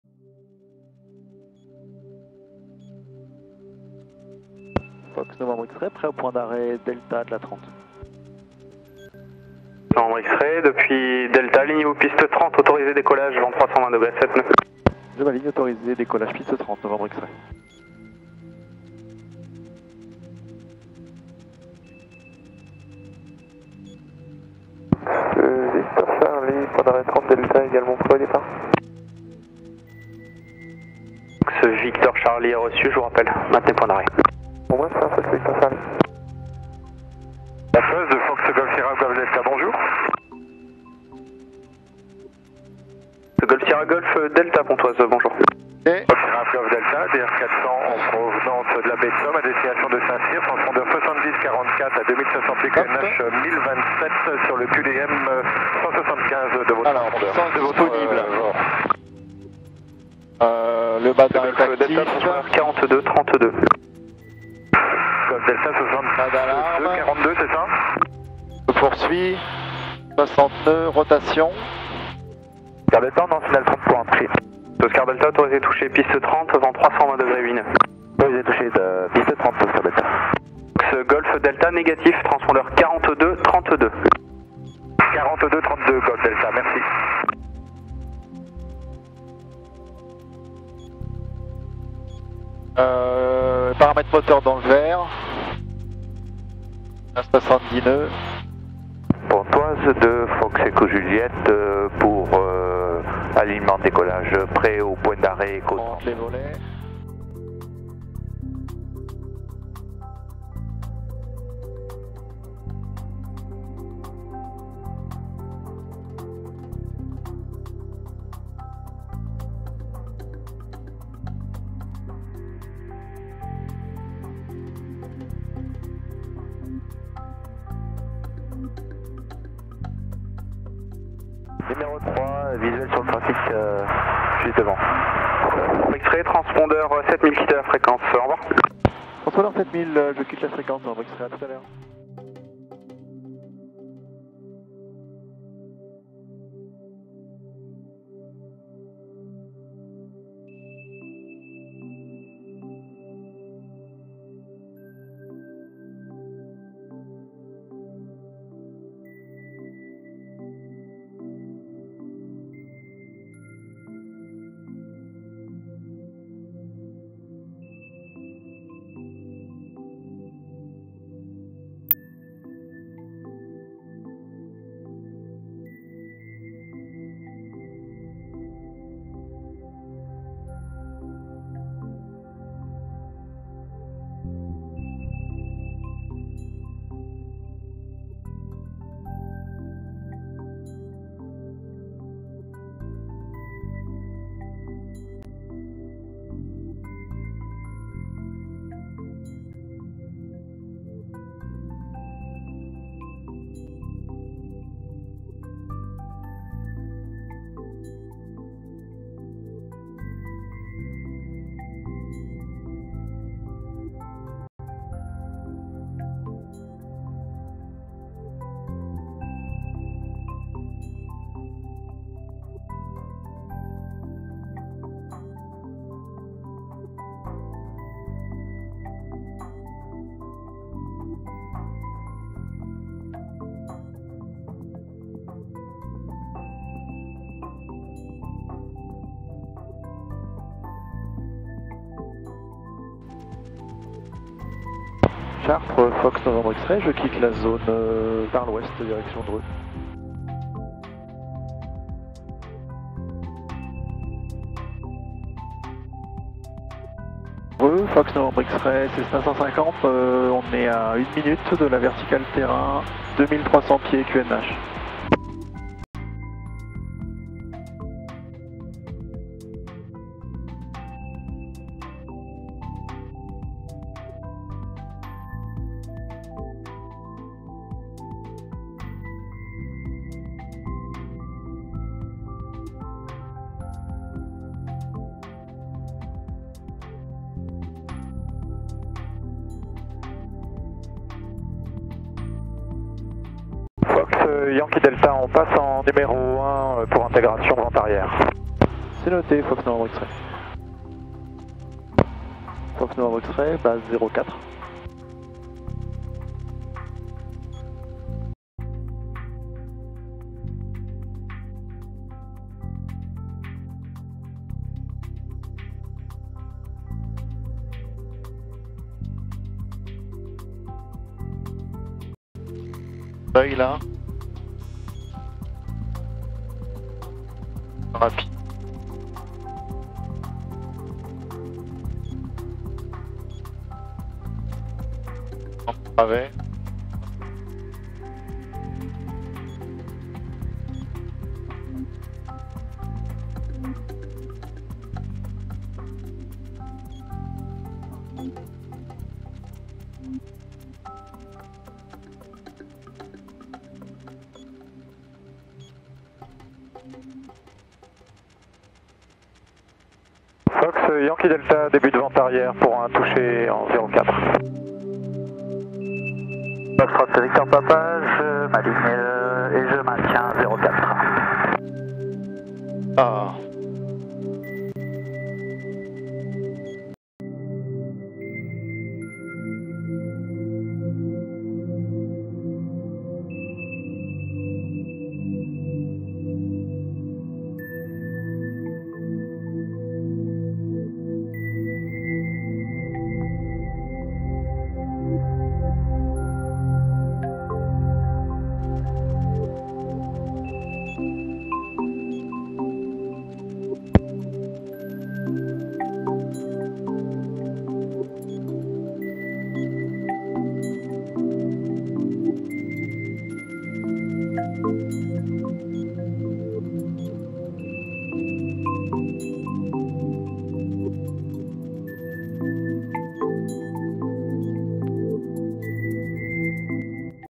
Pontoise Chartres Dreux avec ATC
Navigation de Pontoise à Dreux avec un crochet par Chartres